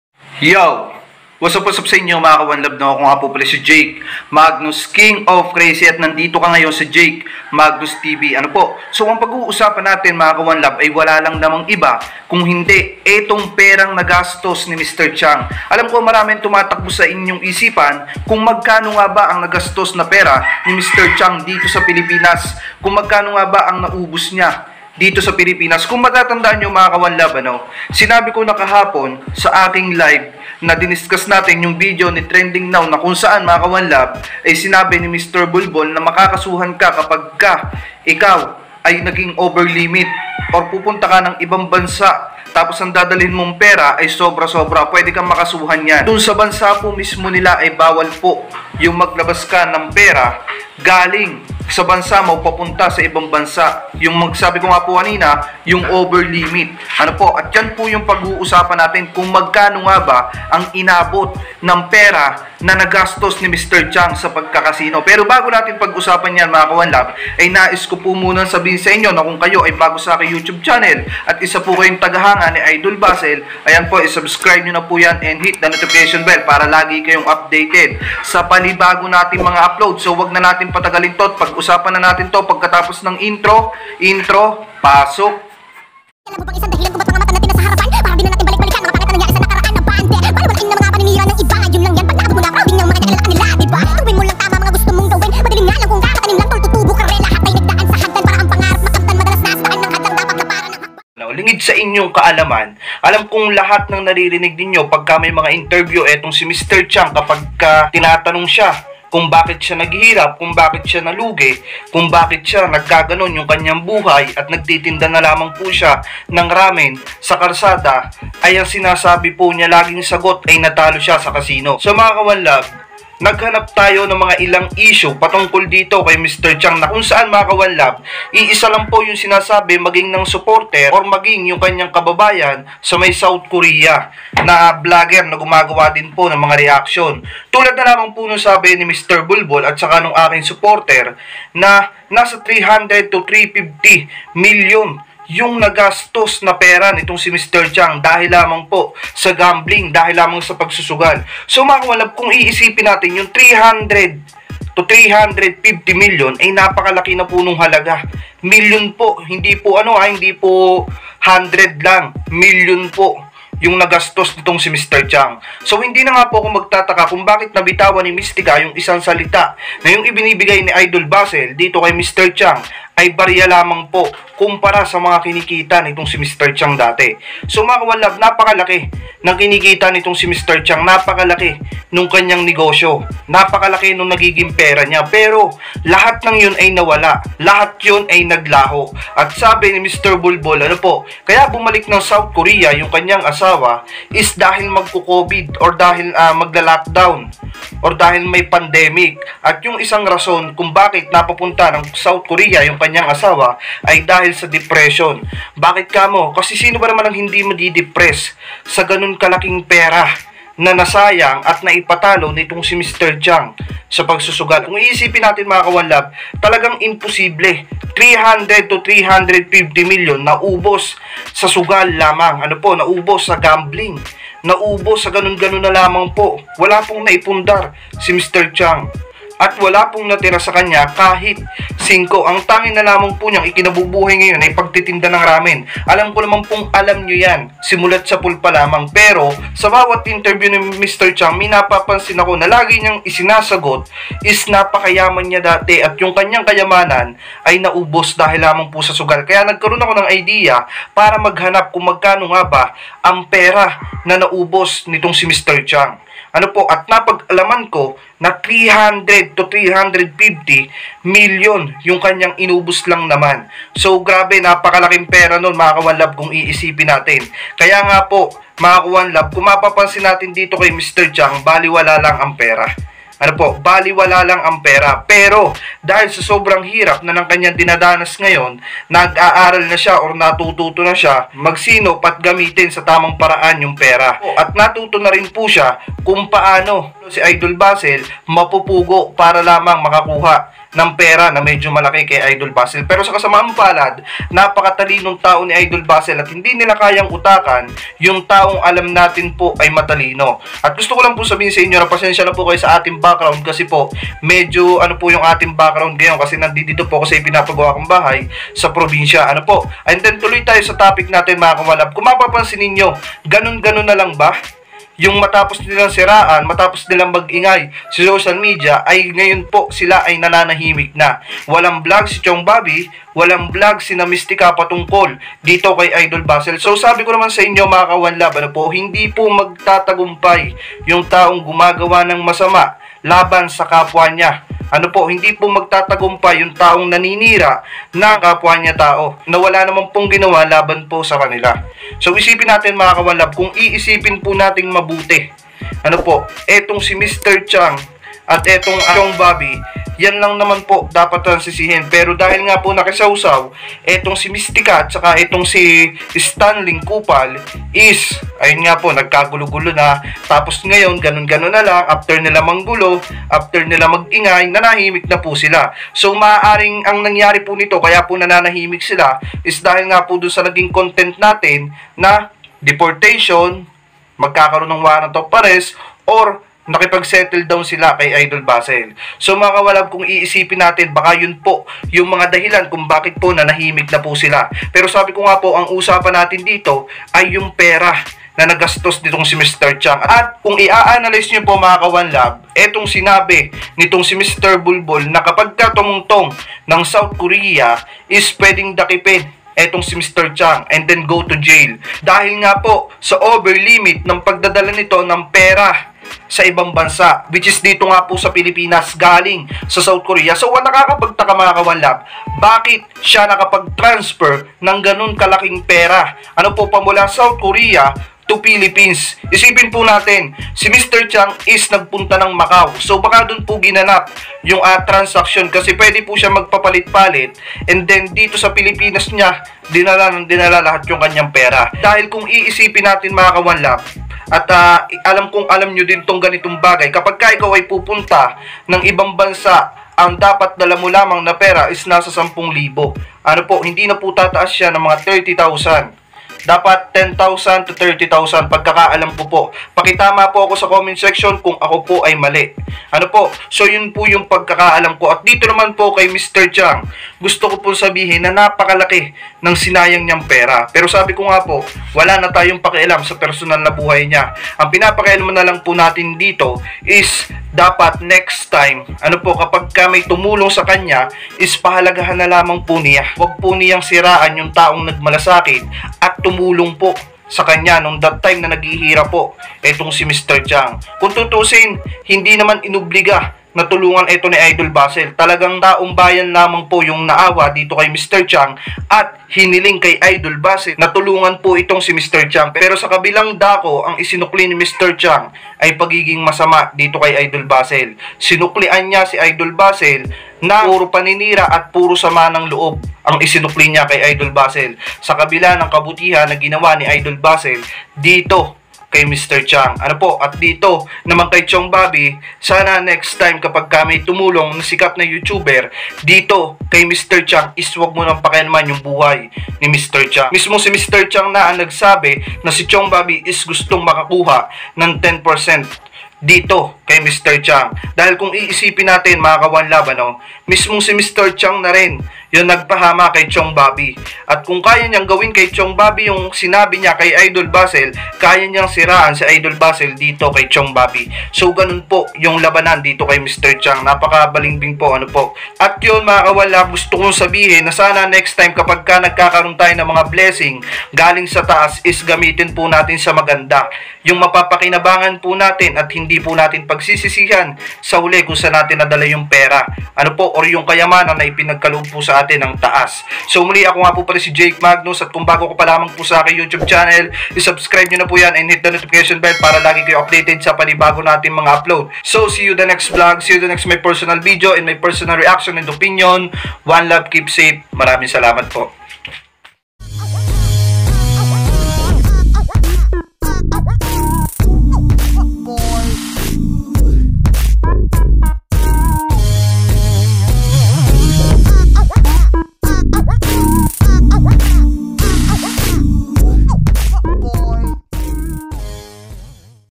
🔴GRABE ITO PALA ANG ISANG DAHILAN KUNG BAKIT UMUWE ANG ASAWA NI MR.CHANG SA SOUTH KOREA DAHIL DITO
GRABE PALA ANG LAKI NG PERANG INUBOS NI MR.CHANG SA CASINO 300M TO 350M NAKAKAWINDANG pero bago natin pag isapan yan wag po natin kalimutan ang salitang always pray and share the blessing because GOD is always good all the time...